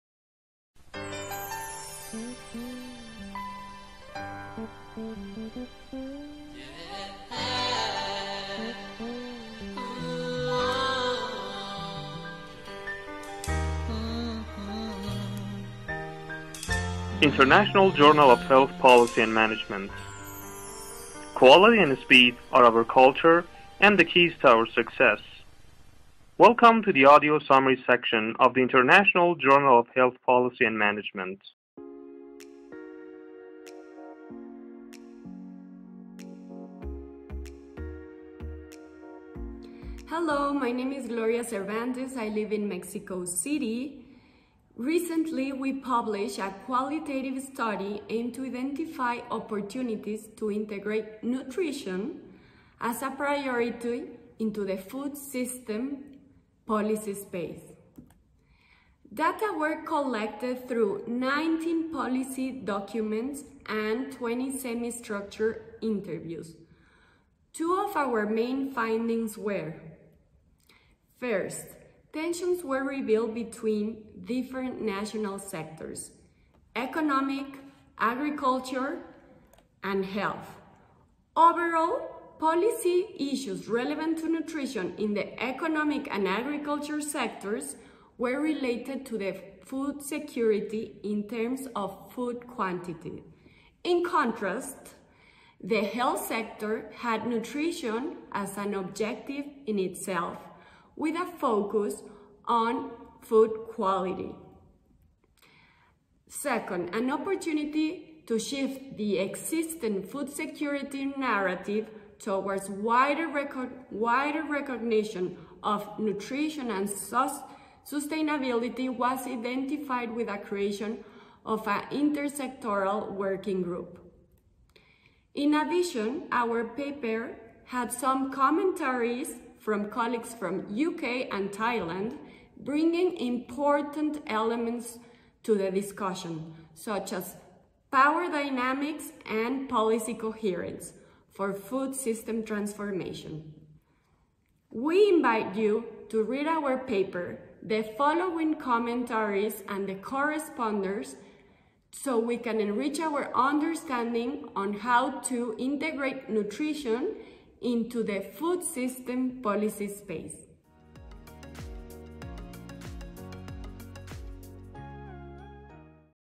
What Opportunities Exist for Making the Food Supply Nutrition Friendly?
Comments: This videocast has been prepared by the International Journal of Health Policy and Management (IJHPM) Editorial Office
Contributing artists: Ali-Akbar Haghdoost, Fatemeh Molaie, Mohammad Tasavon Gholamhoseini
Speaker: Gloria Cervantes
Mixed by: Mahboubeh Khorshidi
Title of paper: What Opportunities Exist for Making the Food Supply Nutrition Friendly? A Policy Space Analysis in Mexico
Authors: Gloria Cervantes (1); Anne-Marie Thow (2); Luis Gómez-Oliver (3); Luis Durán-Arenas (4); Carolina Pérez-Ferrer (5,6)
1) Master’s and Doctorate Program in Medical and Health Sciences, Faculty of Medicine, National Autonomous University of Mexico, Mexico City, Mexico
2) Menzies Centre for Health Policy, School of Public Health, The University of Sydney, Sydney, NSW, Australia
3) Faculty of Economics, National Autonomous University of Mexico, Mexico City, Mexico
4) Department of Public Health, Faculty of Medicine, National Autonomous University of Mexico, Mexico City, Mexico
5) Center for Research in Nutrition and Health, National Institute of Public Health, Cuernavaca, Mexico
6) National Council for Science and Technology (CONACYT), Mexico City, Mexico
This is an open-access article distributed under the terms of the Creative Commons Attribution License which permits unrestricted use, distribution, and reproduction in any medium, provided the original work is properly cited.
Type: MP4
Length: 2:47